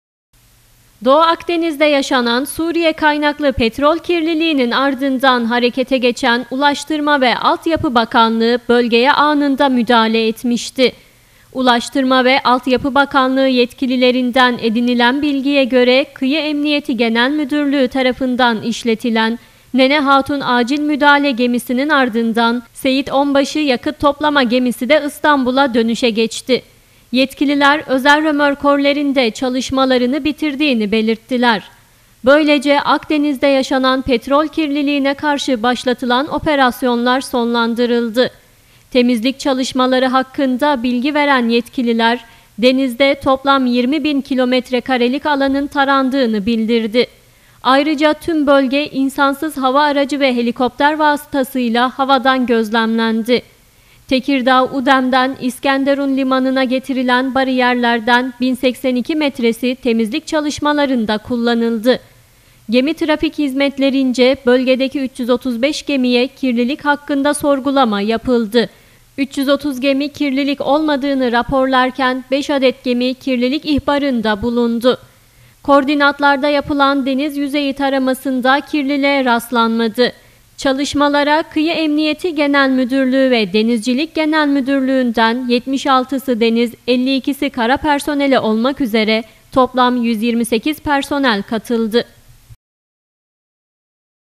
SURİYE’DEN DENİZE SIZAN PETROL, MERSİN SAHİLLERİNE ULAŞTI
Suriye’de bulunan bir termik santralden 24 Ağustos tarihinde denize sızan ve 15 bin ton olduğu bildirilen petrol, Doğu Akdeniz’deki birçok sahile ulaştı. Büyükşehir Belediyesi Çevre Koruma ve Kontrol Dairesi ekipleri, Tarsus plajında temizlik çalışması yaptı.